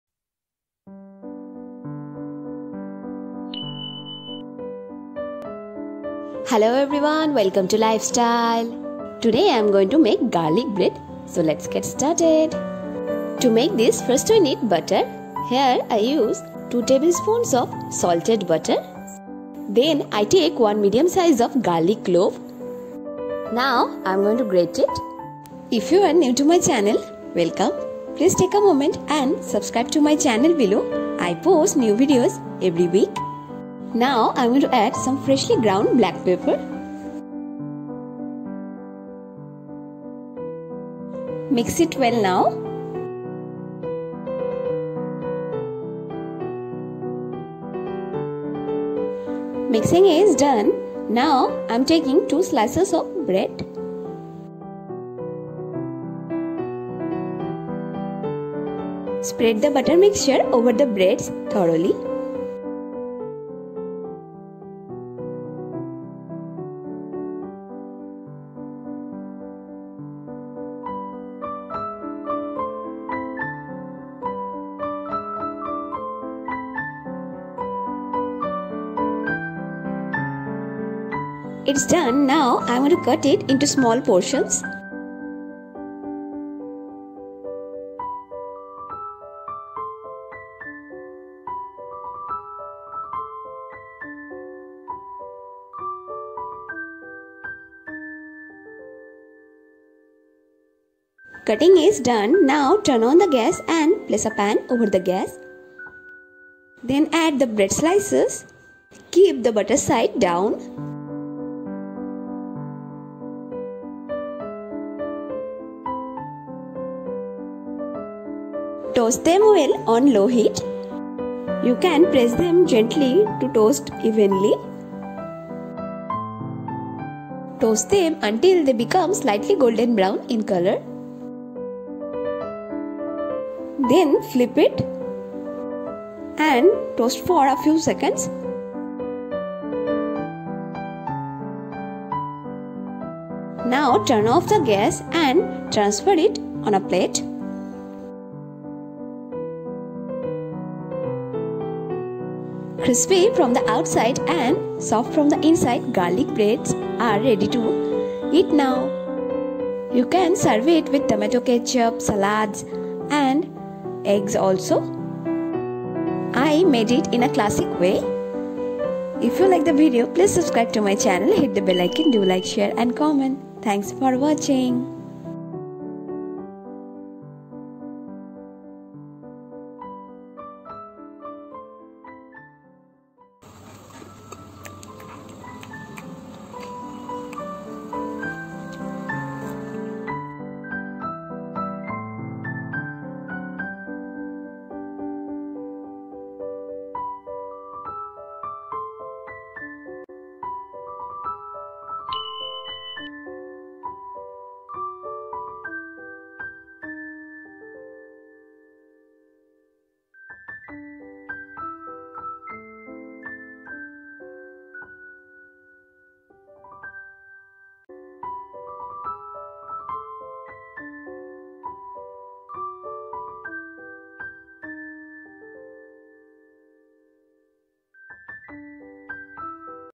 Garlic Bread|Garlic Bread Recipe|Classic Garlic Bread Recipe|Breakfast Recipe|Nasta Recipe
Hello everyone! 😊
Welcome to Life style.
Today I'm going to make classic garlic bread, very easy to make and a quick breakfast recipe.

 for more videos you can watch